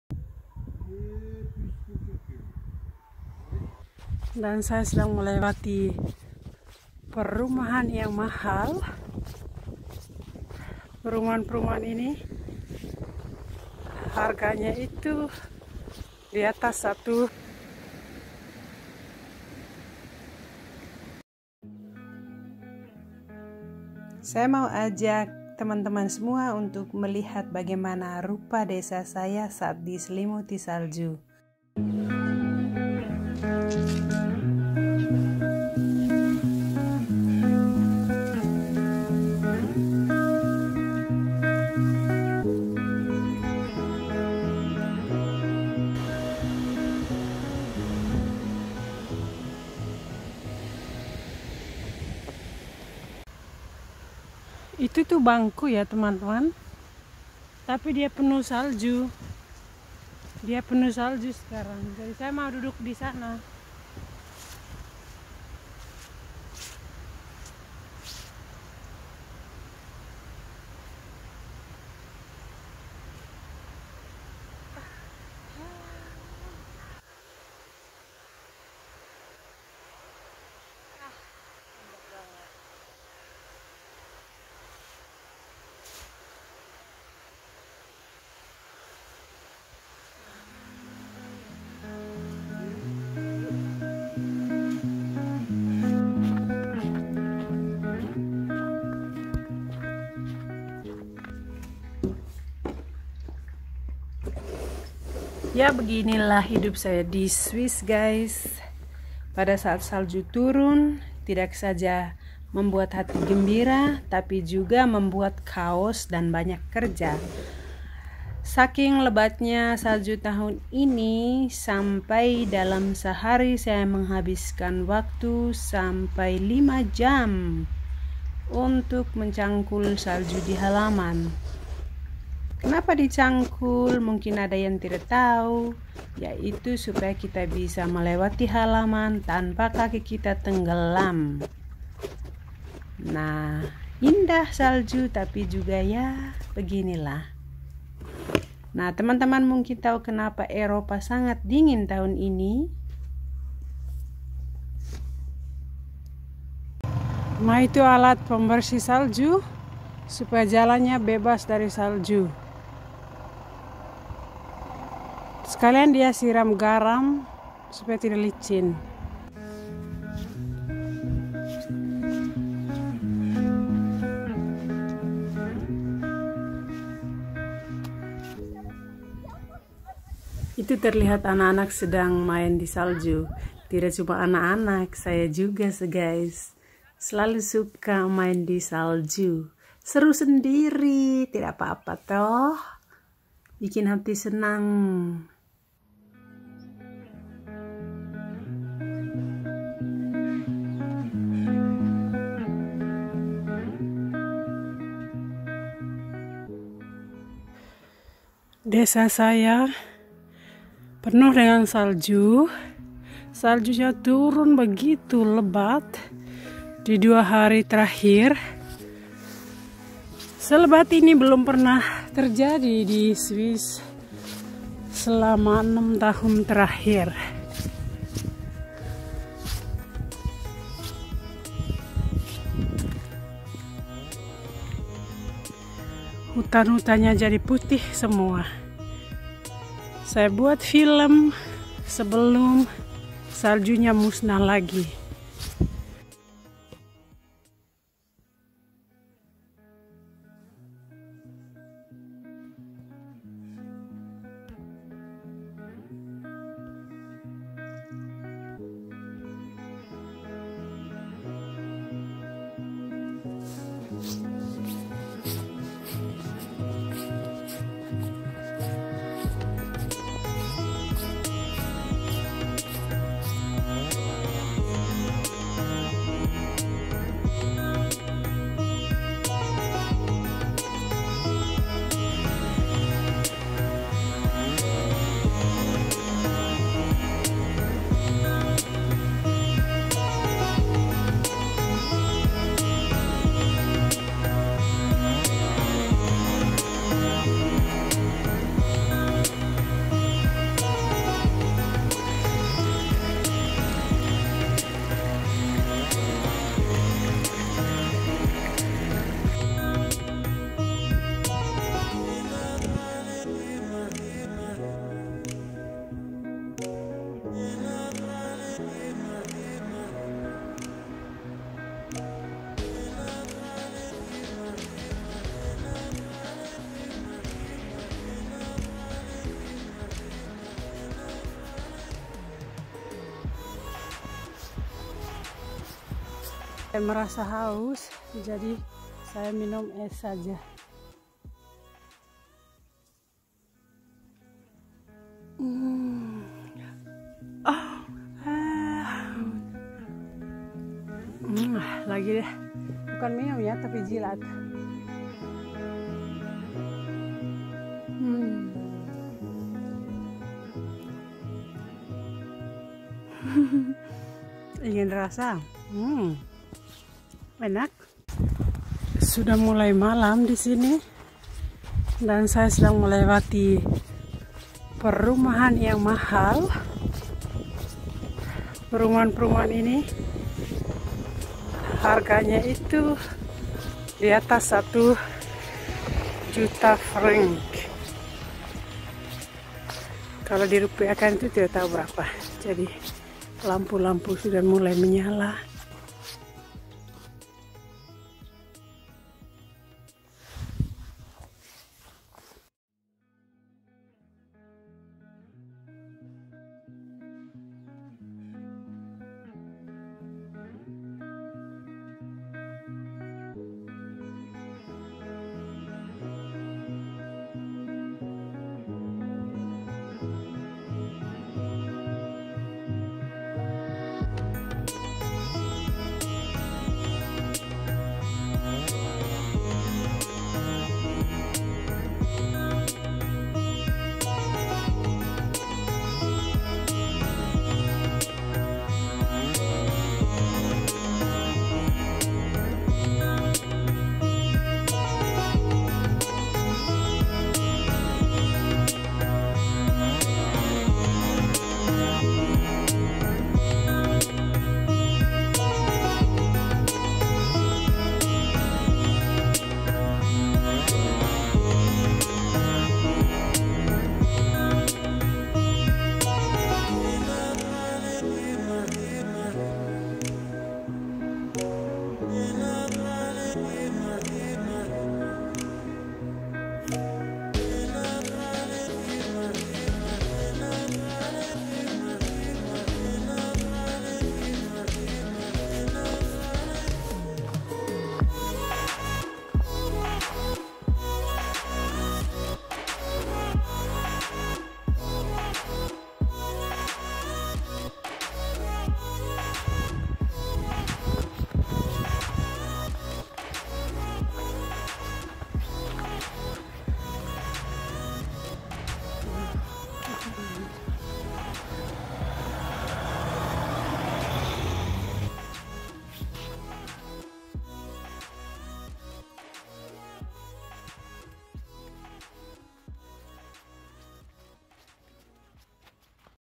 Desa di Swiss saat salju turun
beginilah rupa desaku saat musim dingin.
Thn ini salju turun sgt lebat. Salju sebanyak ini turun cuma 3 hari tapi tebalnya hampir satu meter.
Salju tdk saja buat senang tapi juga bikin repot.
boleh di bilang anak2 happy orang dewasa kesal .karena utk menyetir dan jalan berbahaya bisa tergelincir .